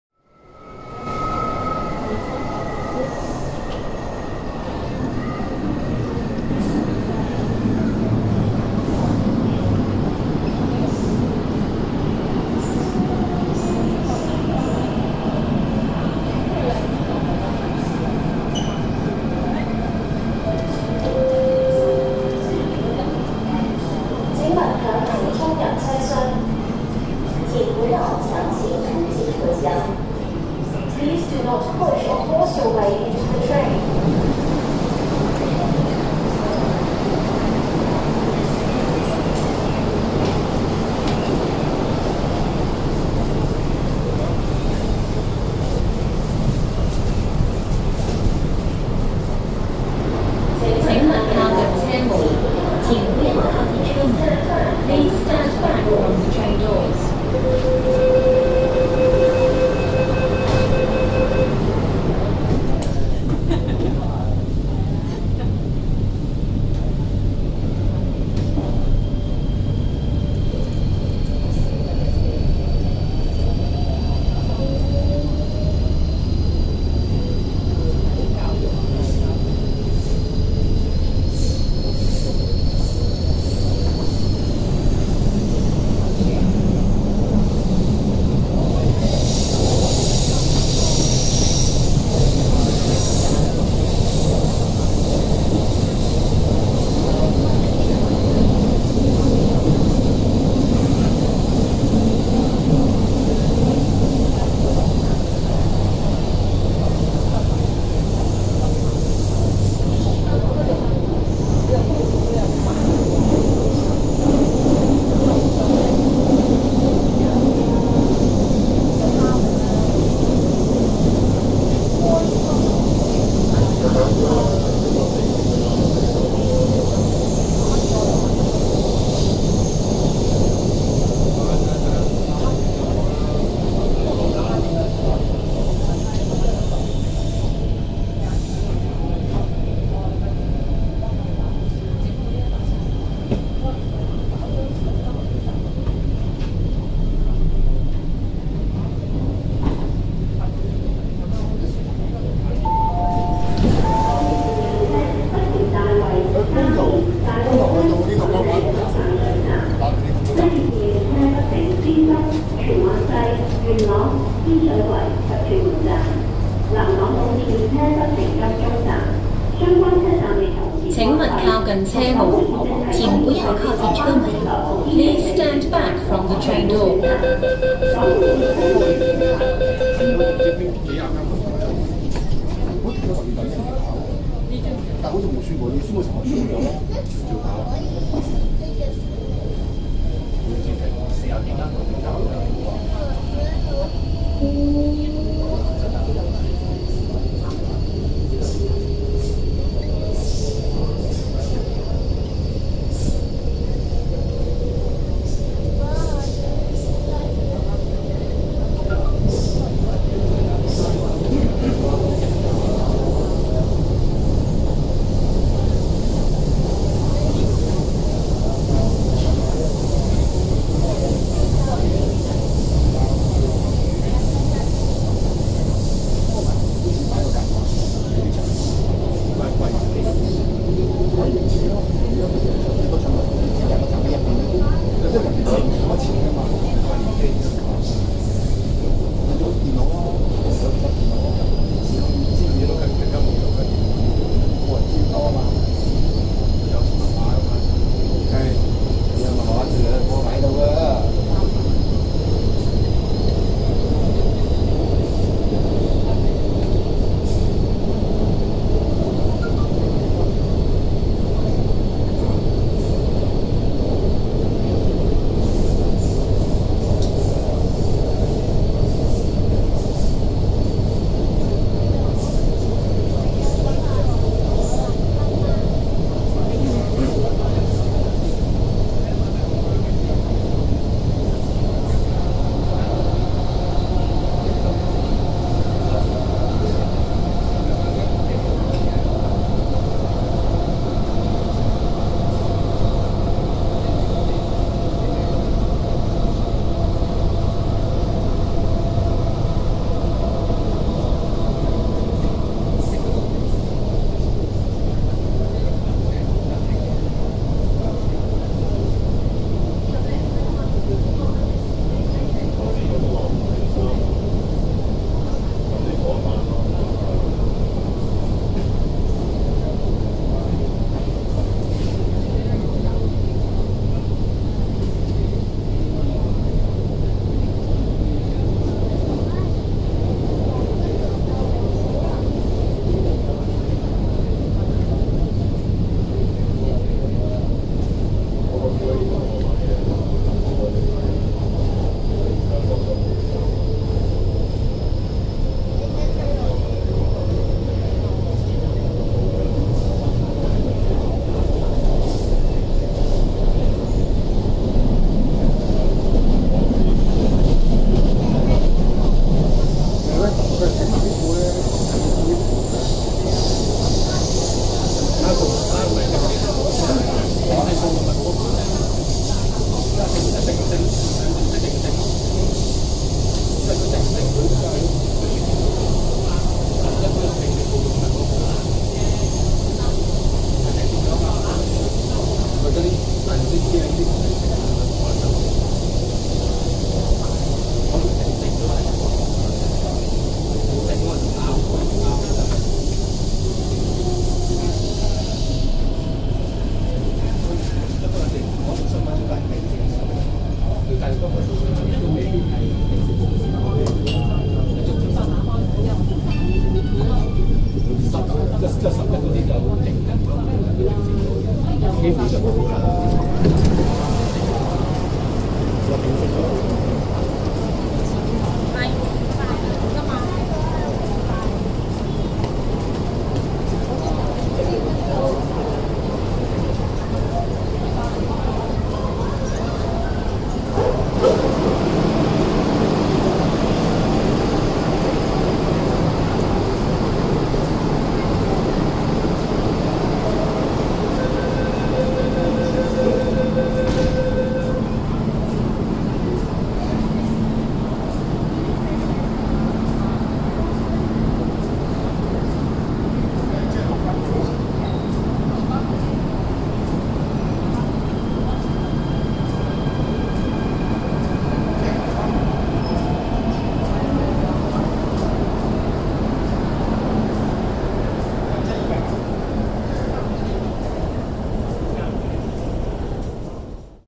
【💥特別服務】不到將軍澳的將軍澳綫 Tseung Kwan O Line（北角－油塘）
片段在2019年10月6日錄製。
港鐵表示部分車站受到破壞，不能使用，列車將會通過車站。在10月6日，有一半的港鐵站關閉，將軍澳綫只維持過海服務，不前往將軍澳的車站。當時列車只停北角，鰂魚涌及油塘站。
According to MTR, some station was vandalised, which trains will not call at the vandalised station. This cause the Tseung Kwan O Line train to only run trains on the cross-harbour section. The train I took only calls at North Point, Quarry Bay and Yau Tong.